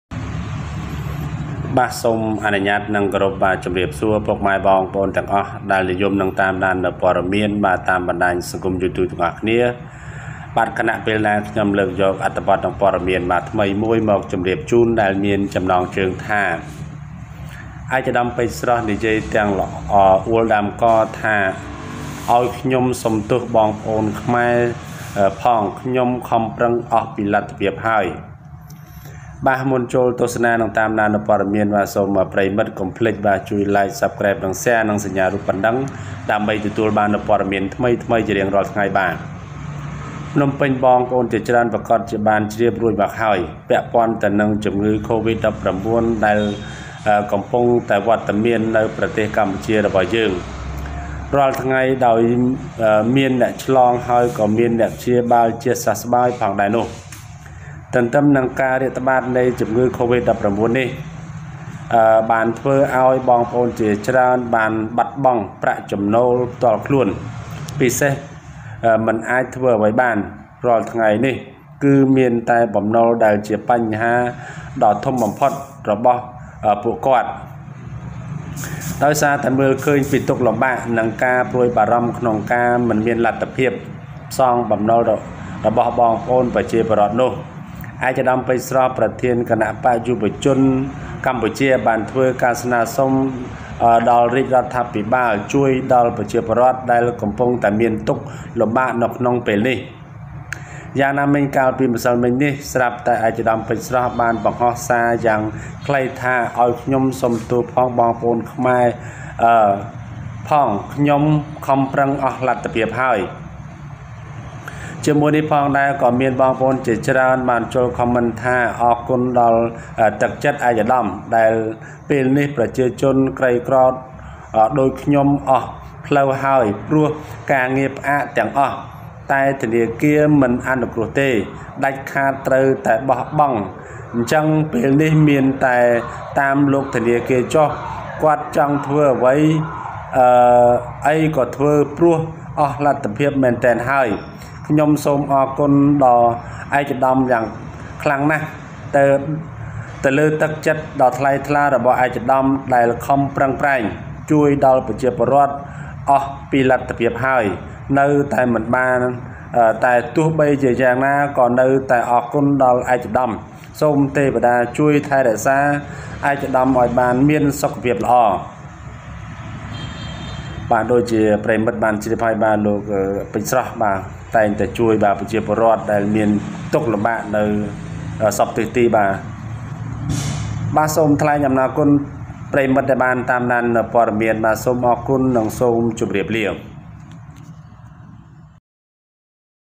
ឯកឧត្តមពេជ្រ ស្រស់ និយាយទាំងអួលដើមករថា ឲ្យខ្ញុំសុំទោសបងប្អូនខ្មែរផង ខ្ញុំខំប្រឹងអស់លទ្ធភាពហើយ...
His Excellency Pich Fresh said, "I apologize to the Khmer people, I try my best ..."